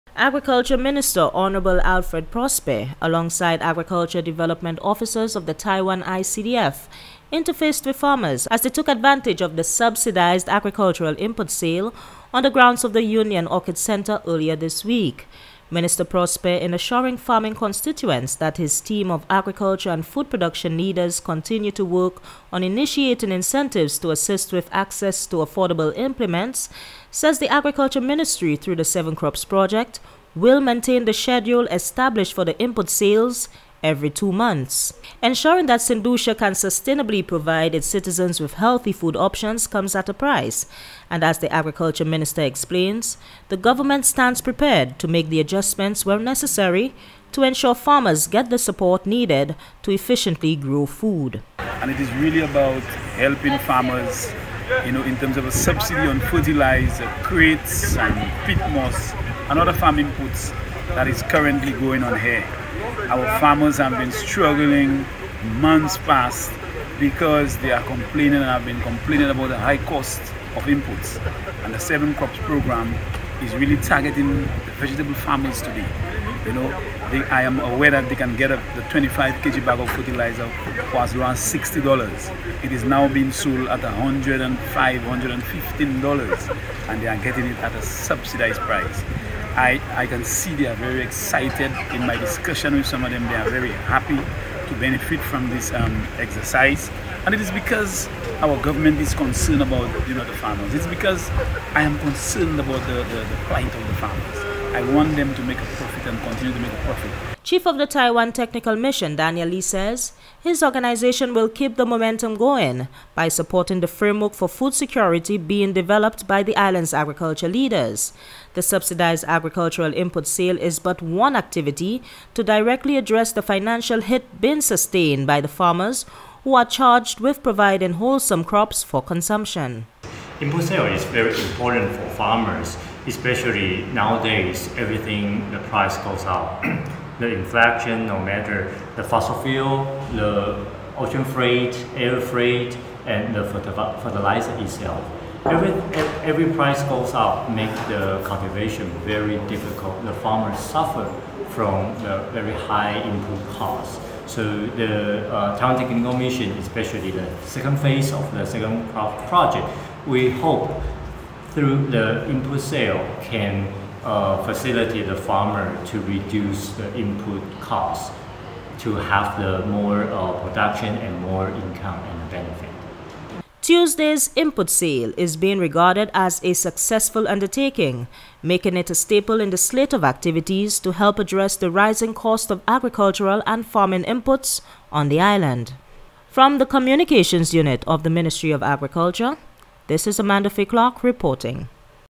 Farmers benefit from subsidized sale of agriculture products
Farmers gain reprieve to the rising cost of agricultural and farming inputs, through a subsidized agri-inputs sale. This, as efforts intensify to support food security in Saint Lucia.